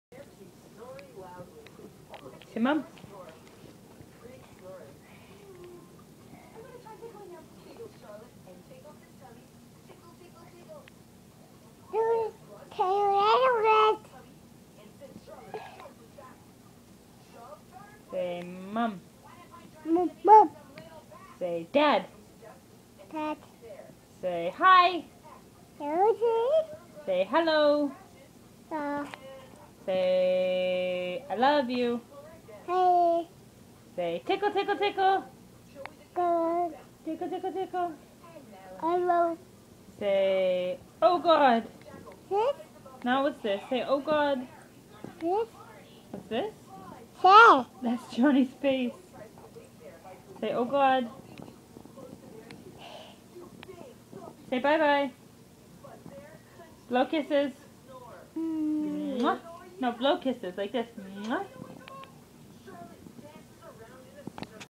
A few more words
Johnny shows off a few of his words